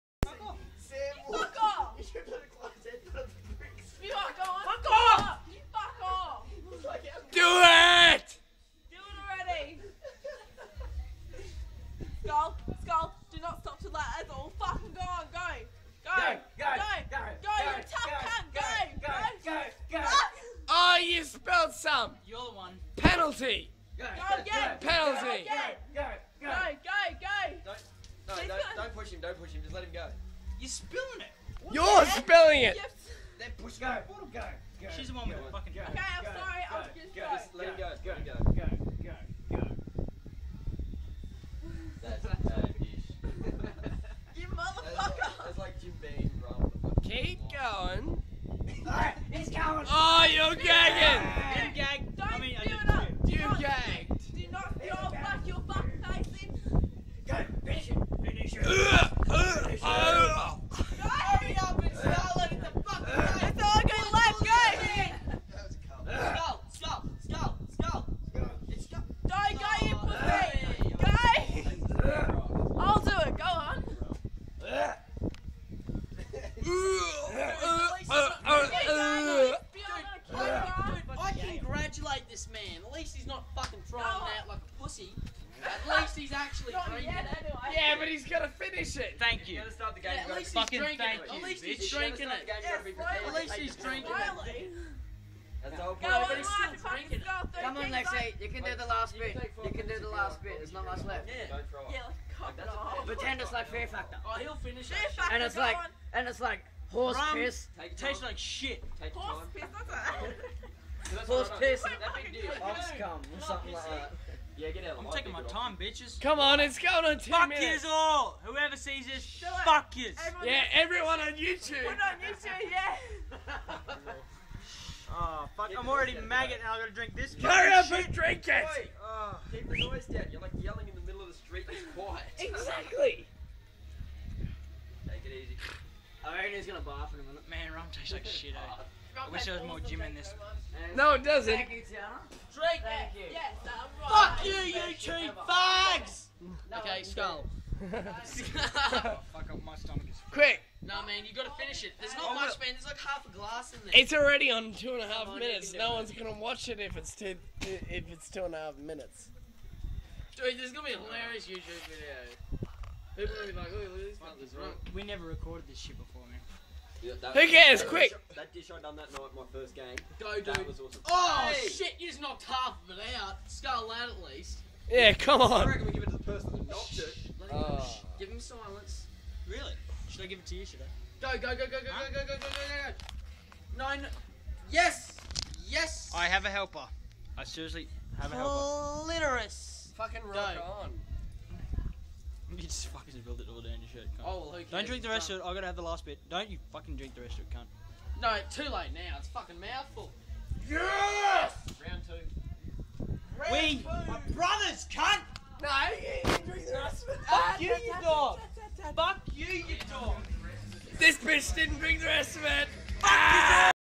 Results of drinkin games...
This guy lost a drinkin game and takes forever to finish a bowl of rum and coke... so i had to come and finish it for him hehe... and please excuse them, youll see why!